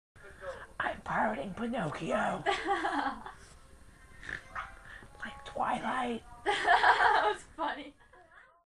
A Funny at AF's House
We were Watching a Movei and I was Randomly Recording it and like ,most other times, I caught something rare and funny. Watch it to find out what it is? If Lost just ask me. ^w^